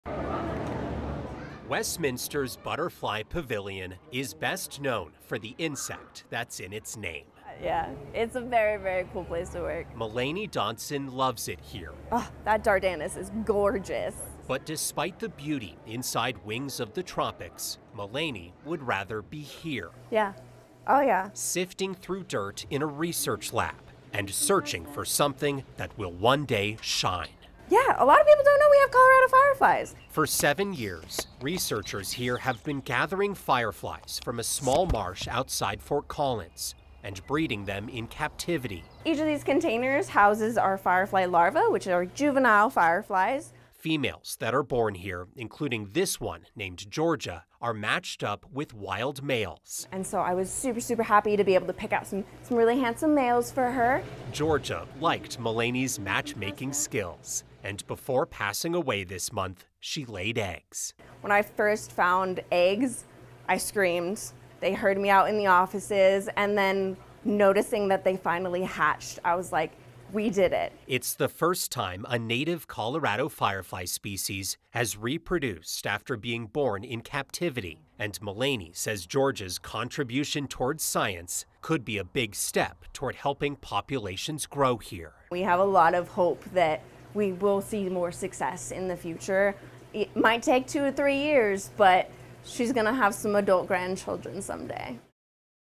Butterfly Pavilion first to complete lifecycle of a Colorado firefly under human care
Butterfly Pavilion said this achievement sets a new standard for firefly conservation and invertebrate research worldwide.